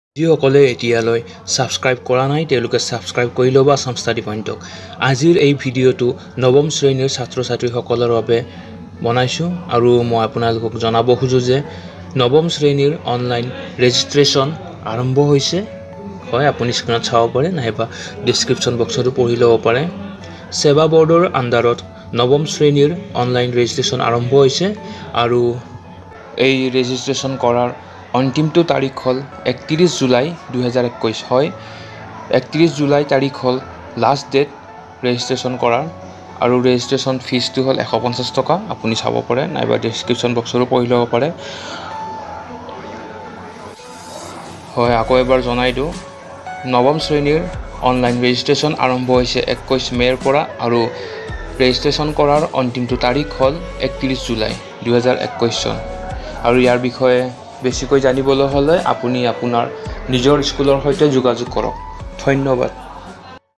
SEBA Online Registration 2021 | Class 9 online Registration 2021 | নৱম শ্ৰেণী
SEBA Online Registration 2021

Board of Secondary Education, Assam (SEBA), the official state level board of Assam usually employs online registration process for registering class 9 students under it. This year too, SEBA activates its online registration portal for the registration of class 9 (IX) students. All students under this board must complete their registration by 31st July 2021.

SEBA Online Registration Details

Name of process: Online Registration

Name of standard/ class: Class IX

Name of academic session: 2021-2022

Name of academic board/ council: Board of Secondary Education, Assam

Name of class: Class IX (9th)

Academic session: 2021-22

Registration fee: Rs.150 /- (To be paid online/ challan)

Last Date: 31st July 2021

How to Complete SEBA Registration Process?

The school authority is already provided with all required login credentials through which they can complete the registration process. In case of lost credentials, they may contact respective member secretary for concerned district/s.

Important Instructions for filling up the SEBA Registration Form

# Please enter Student's Name, Father's Name, Mother's Name and Date of Birth correctly as per the School Admission Register (SAR) and Birth Certificate.

# The age of the registered student should be minimum 13 years as on 1st March, 2021.

# For any query please contact Member Secretary (DPO)/EDP of the Regional Office of SEBA.

# The User Name and Password of school authority will remain same as of last year. However, the newly converted schools, User Name and Password will be provided by the Inspector of Schools.

Important Dates of SEBA Registration 2021

# Starting date of online registration: 21st May 2021
# Last date of online registration: 31st July 2021